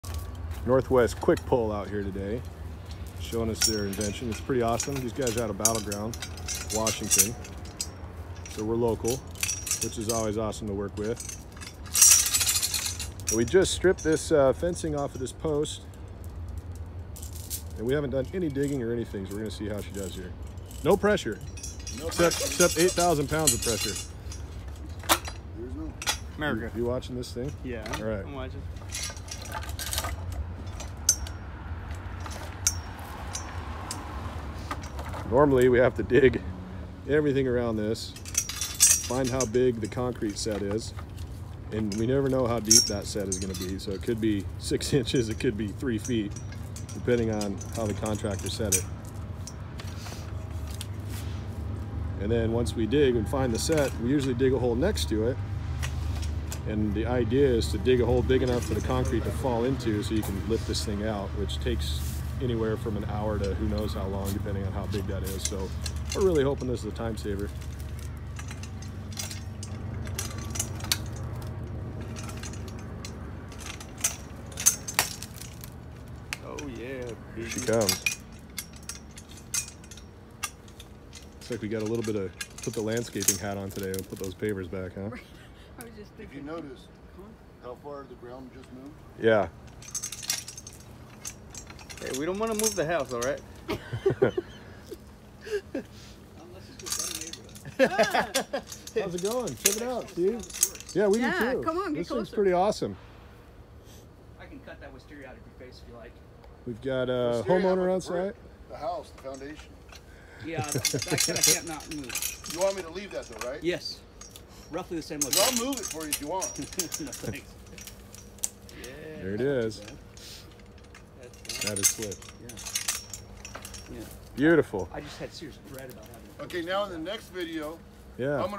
Fence Butlers Try The NW Quik Pull
Local fence installation and repair gurus Fence Butlers give our tool a try for the first time. they bought two on the spot.